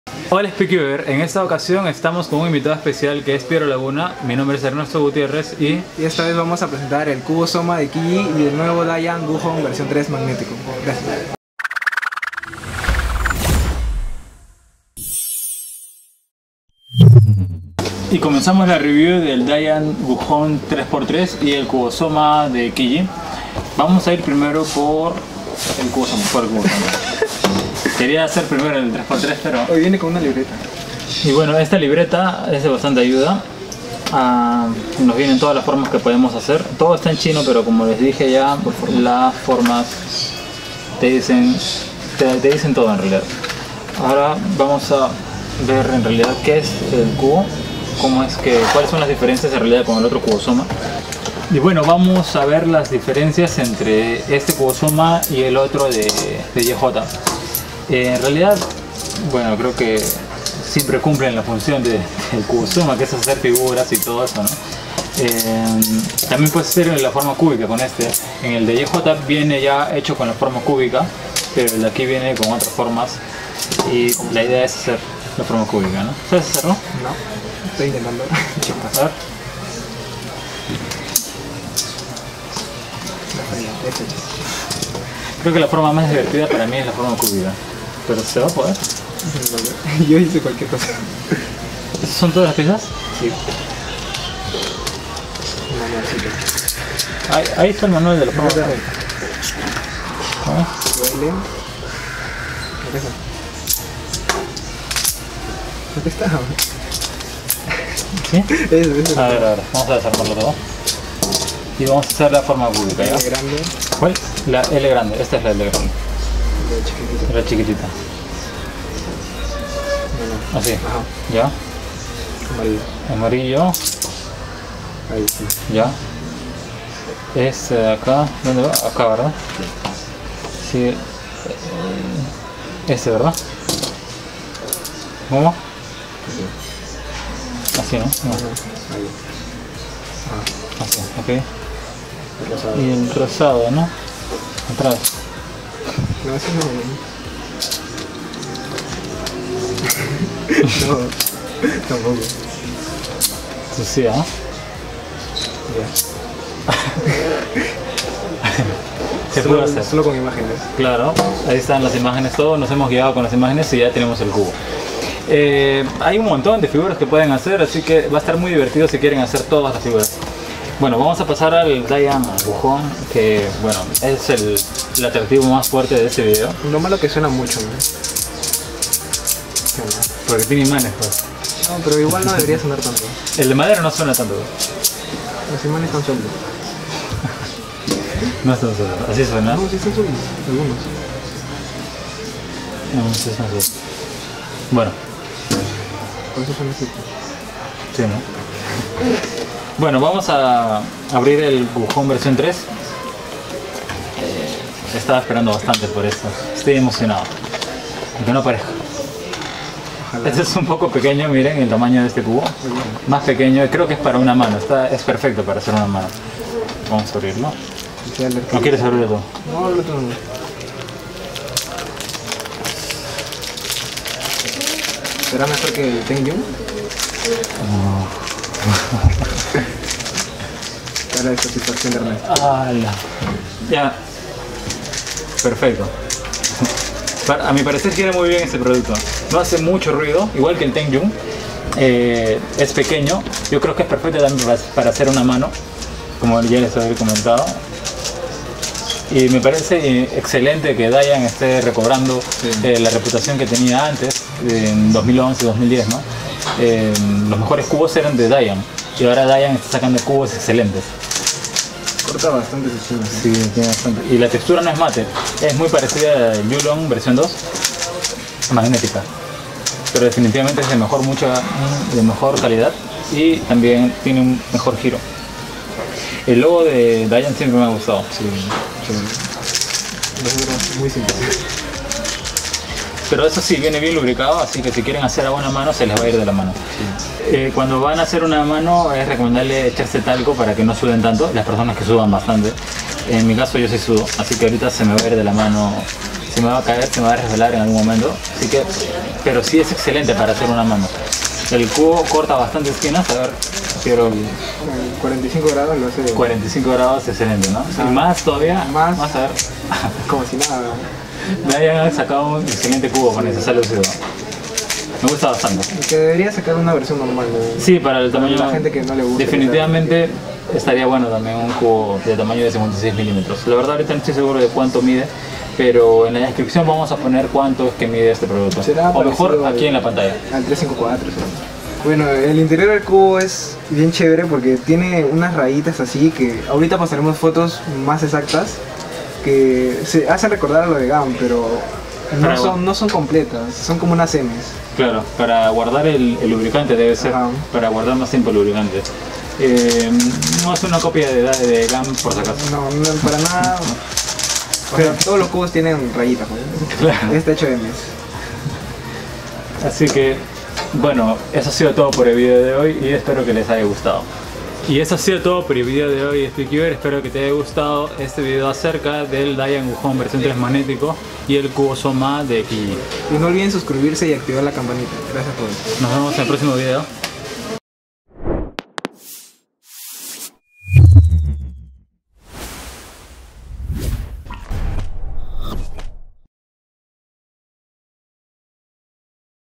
Review Qiyi Magnetic Puzzle – Cubo Soma & DaYan GuHong 3M en ESPAÑOL | Speedcuber Perú
Tu tienda favorita de cubos te trae una review del nuevo cubo soma llegado a la tienda y la nueva versión del DaYan